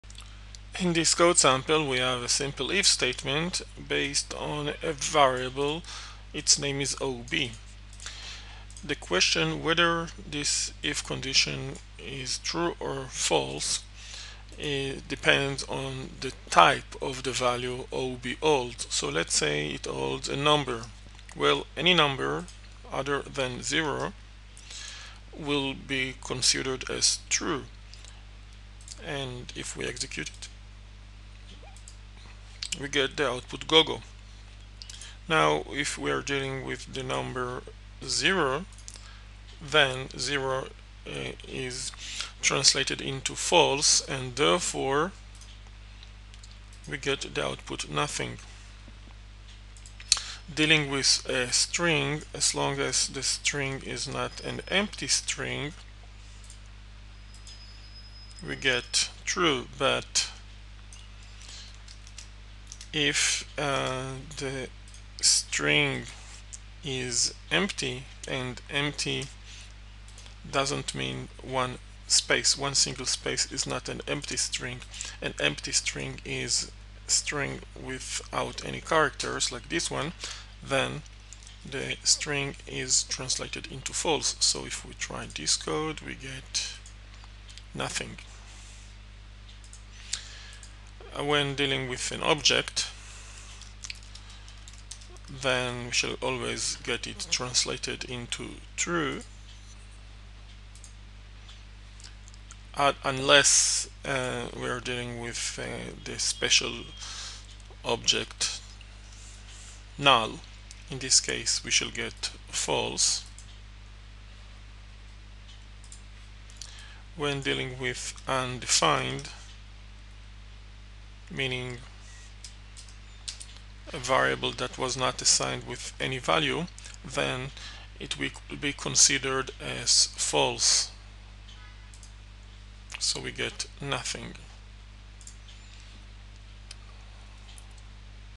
The Boolean Type in Java Script
When a value is converted into boolean getting true or false depends on its value. This video clips explains it using a simple code sample.